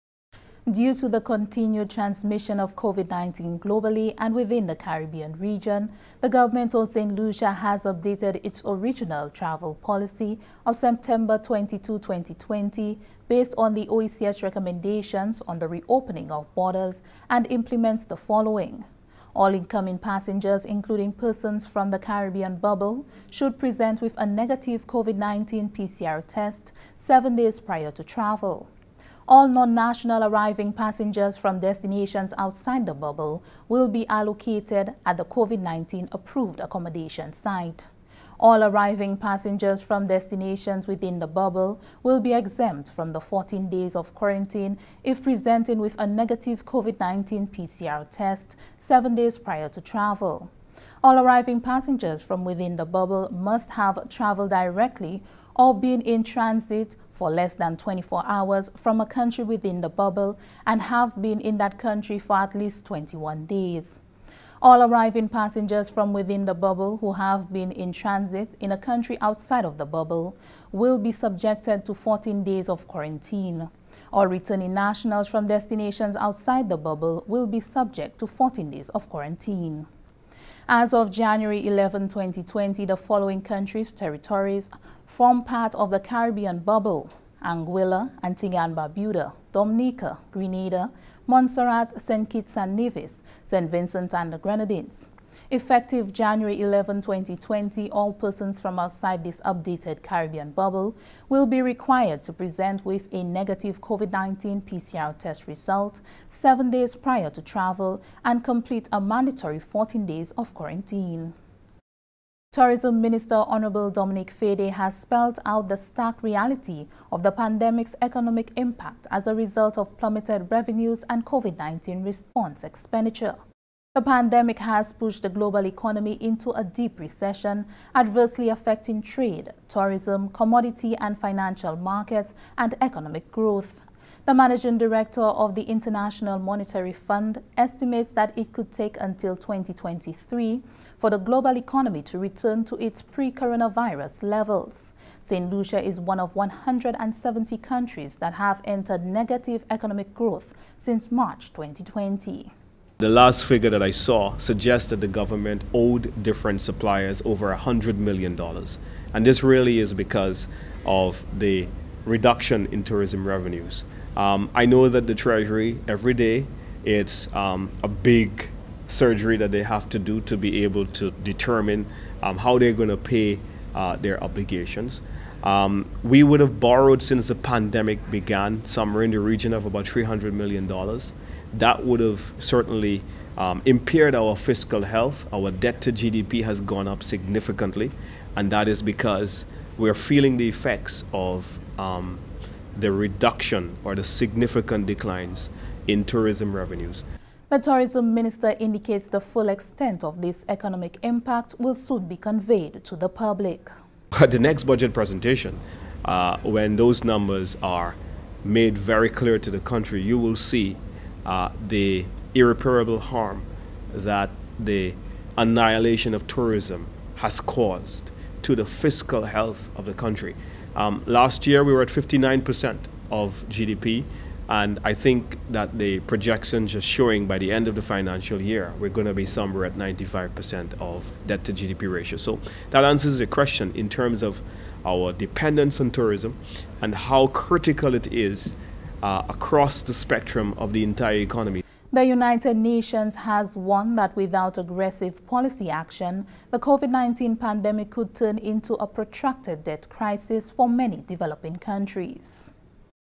Saint Lucia updates Caribbean 'travel bubble' list
Due to the continued transmission of COVID-19 globally and within the Caribbean region, the Government of Saint Lucia has updated its original travel policy of Sep. 22, 2020 based on the OECS Recommendations on the Reopening of Borders.